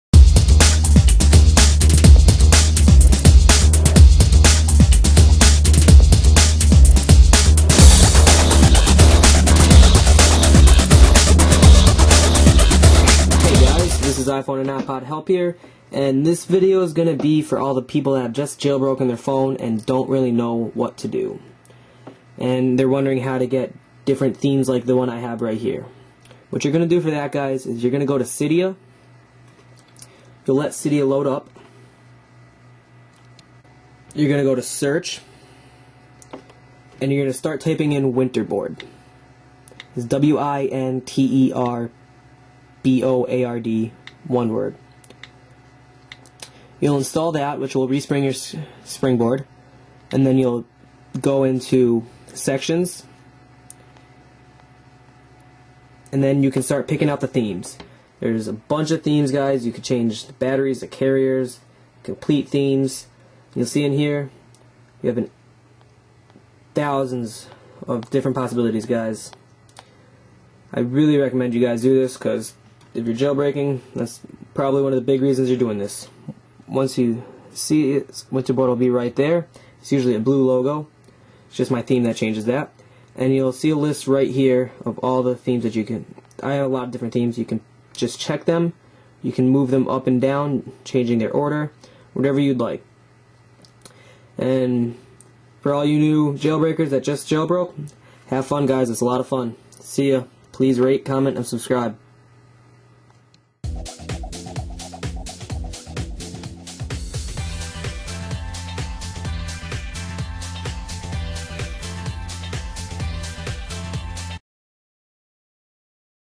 How to Change your theme on your iPhone or iPod Touch with WinterBoard!!
Hey YouTube! This is a video for those of you that have just jailbroken your iPhone or iPod Touch, and are not sure how to alter anything on your device, such as your theme. It is very simple and quick to do!=) Its called WinterBoard and it is found under Cydia! All you do is install WinterBoard(requires respring) and then go to sections, and choose from the thousands of themes, which most have previews!! Have fun modding your iPhone or iPod Touch everyone!!=)

Skype- Joshx413